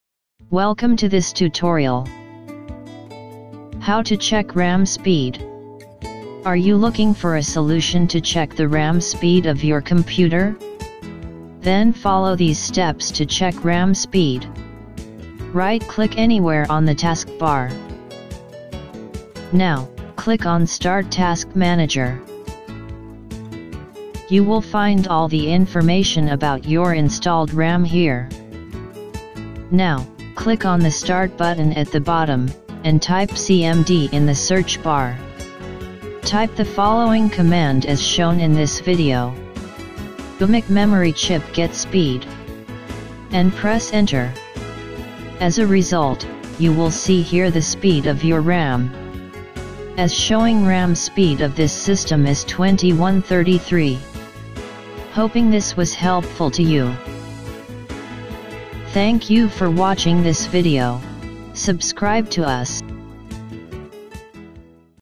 How To Check Ram Speed :Tutorial
Are you looking for a solution to check the RAM speed of your computer? Then follow these steps to check RAM Speed.

1. Right-click anywhere on the Taskbar.
2. Now, click on Start Task Manager.
3. You will find all the information about your installed RAM here.
4. Now, click on the Start button at the bottom, and type CMD in the search bar.
5. Type the following command as shown in this video
wmicmemorychip get speed
and press enter.
6. As a result, you will see here the Speed of your RAM. As showing RAM speed of this system is 2133.

Hoping this was helpful to you.